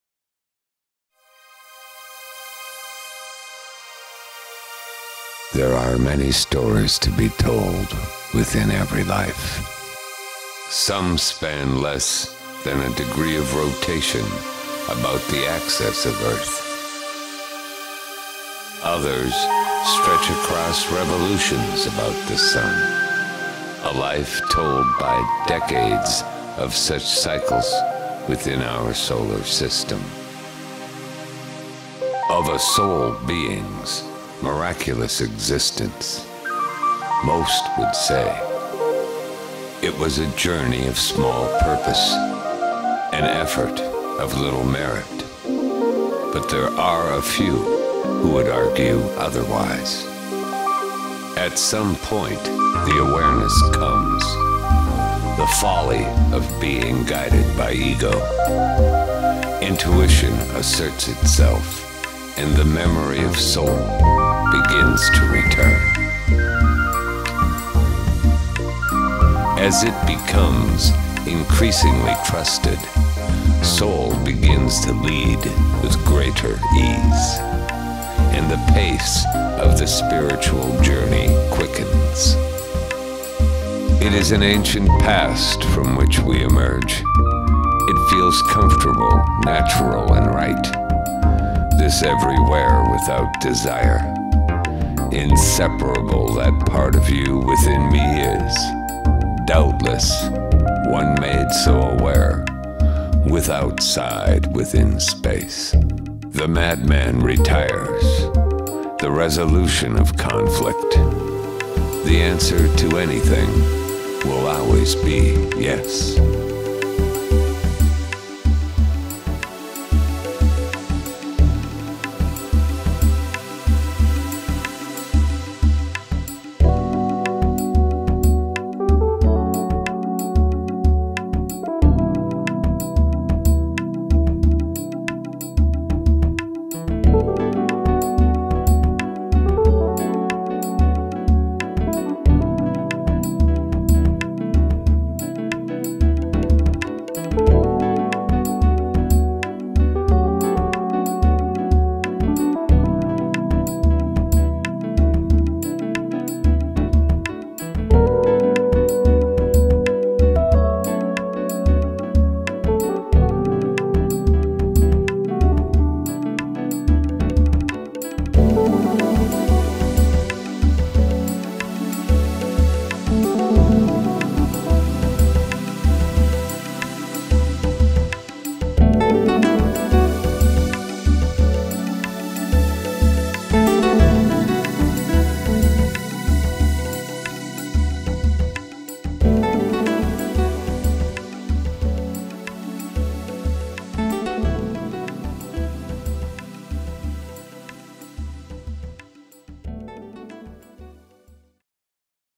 shoes on the table
3.5 min. clip of coffee table book of verse and photography with author narration and music